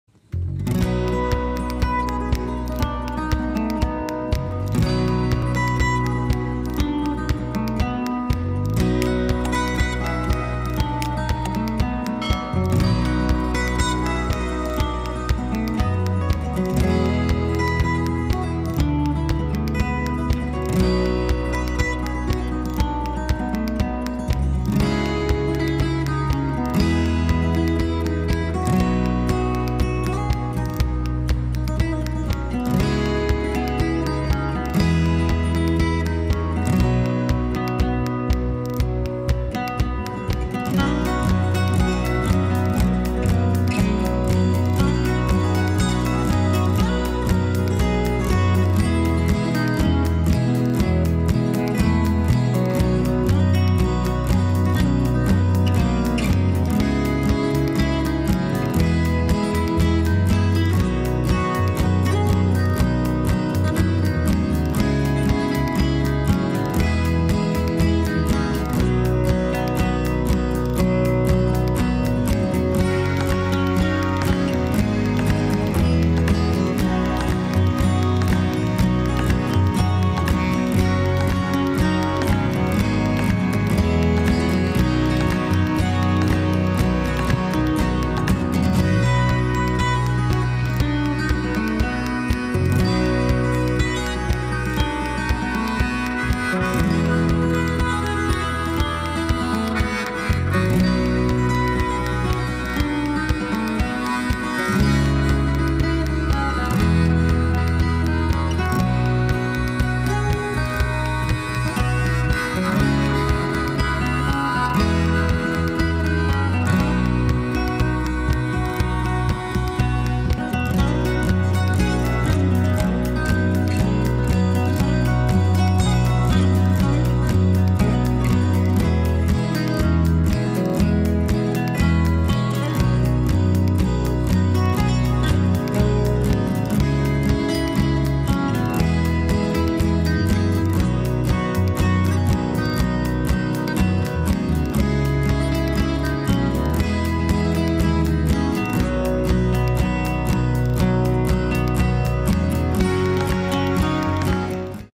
Poland, village nature by drone eye | DLD 0.3
Time on polish village is awesome time to touch nature, how touch nature from fly by drone eye

00:00 - awesome view of polish village
00:17 - polish chicken
00:45 - polish nature
01:12 - flying birds
01:31 - polish fields
02:08 - polish church